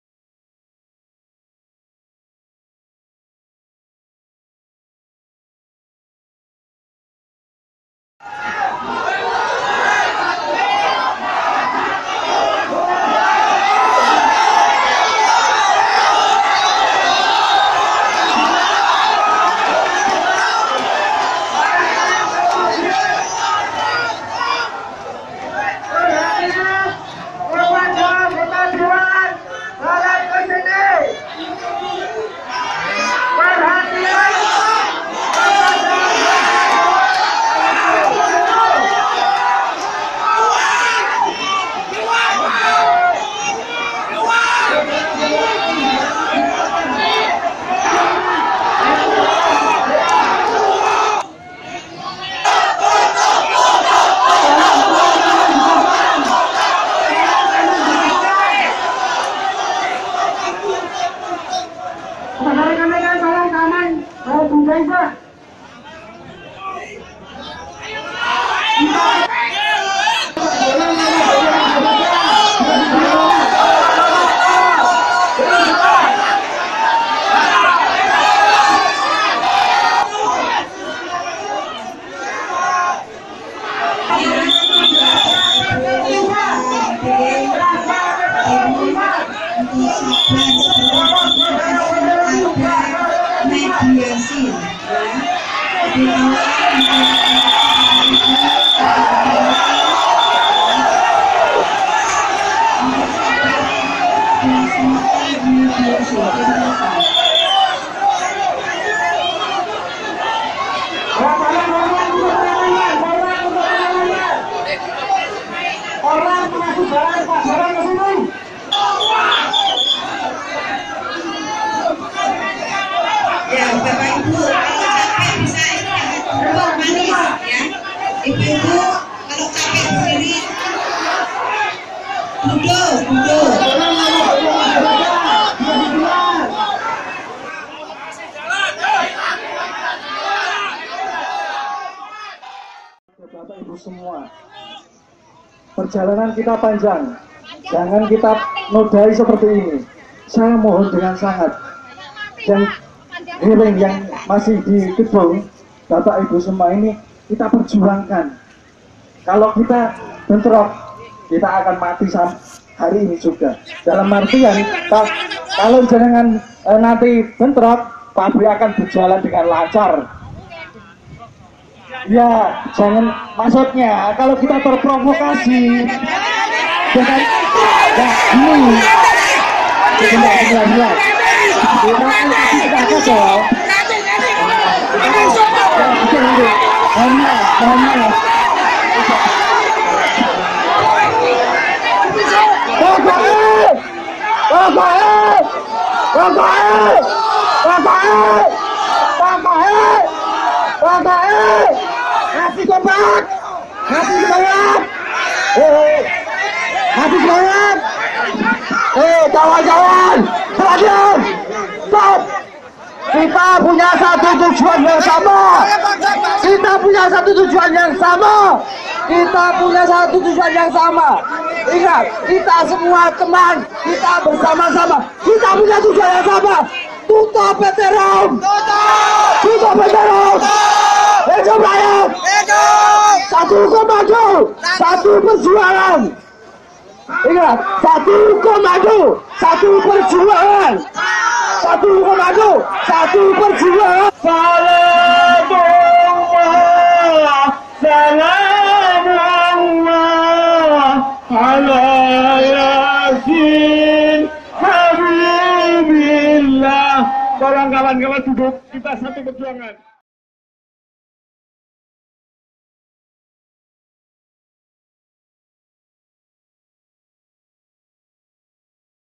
Dikira Ada Penyusup, Demo PT RUM di DPRD Sukoharjo Nyaris Ricuh
Aksi demo ribuan warga dari dari lima desa disekitar PT RUM di DPRD Sukoharjo nyaris ricuh. Massa mencoba merangsek mendekati pintu masuk Gedung DPRD meminta para wakil rakyat segera menemui mereka. Sesorang orator yang mencoba meredam aksi tersebut dituding sebagai penyusup Simak Videonya